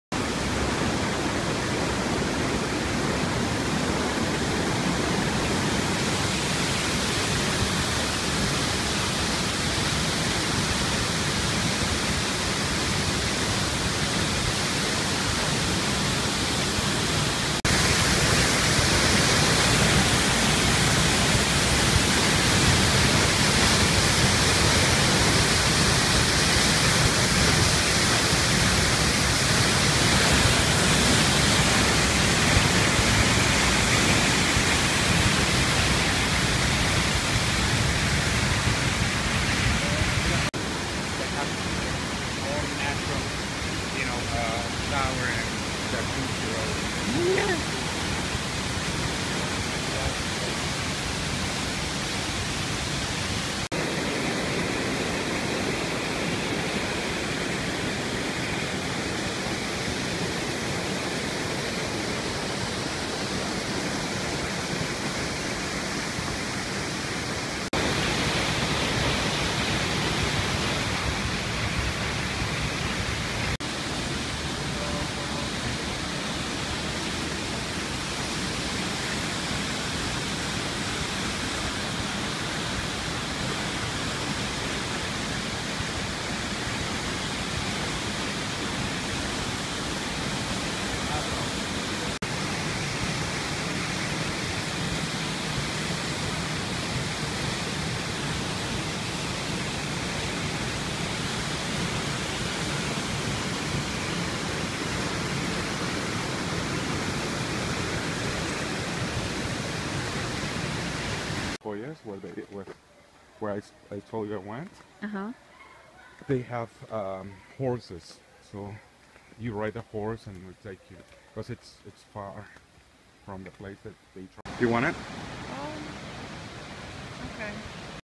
La Paz Waterfall Gardens in Costa Rica
At La Paz Waterfall gardens you will not only experience the raw power of nature but also its softer side. This is home to the largest butterfly observatory in the world